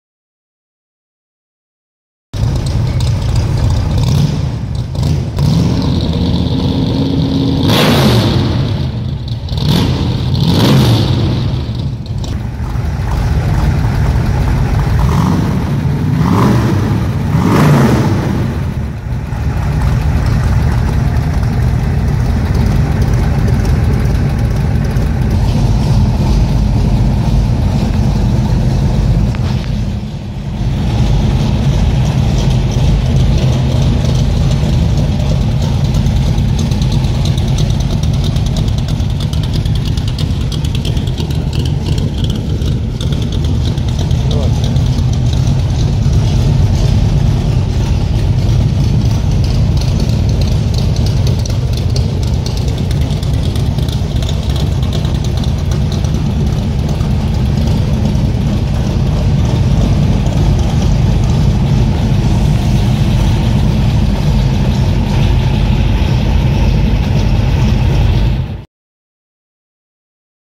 1965 Kirkham Cobra 427 SC for Sale by Master Builder Stewart Hall
to see this 1965 Kirkham Shelby Cobra 427 SC for sale from San Francisco Sports Cars.

This stunningly beautiful Kirkham 427 S/C Cobra adorned with its polished aluminum body with brushed dual stripes is a piece of rolling sculpture. Kirkham #0362 was built by master builder Stewart Hall from Hall Fabrications in Benicia CA. Stewart specializes in restoring and maintaining the original Shelby Cobras as well as other fine exotics on race cars.

The Aluminum Shelby 427 has been stroked to 482 Cu in and was built by Tony Oddo. The carburetor is a Cathedral Bowl as the original. The exhaust headers and side-mounted exhaust are built to original specifications. TKO 5-speed transmission. The suspension is fully independent unequal length A-arms and adjustable coils-over shocks. The wheels are Aluminum six spoke, pin drive Halibrand style as original.

Check out the link above for full description, current pricing info and detailed photo gallery. Give me a call with your best offer.

Brad Goldstone
San Francisco Sports Cars
191 Industrial Rd